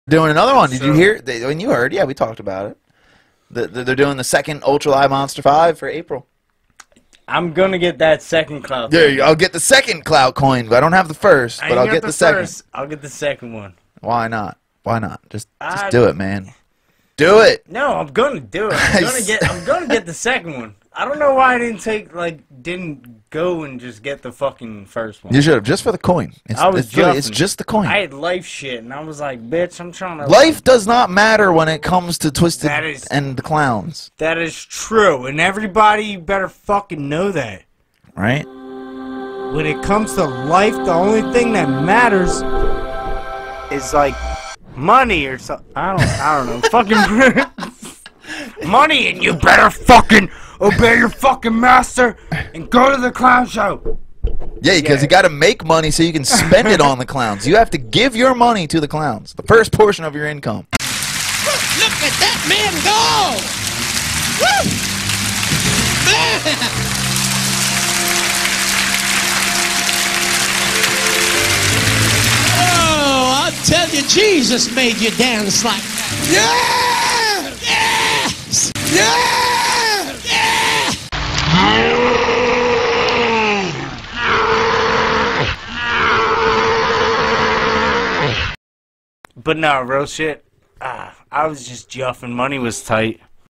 The Lord Helps a Man to Walk
Give God the first portion of your income and he will heal you.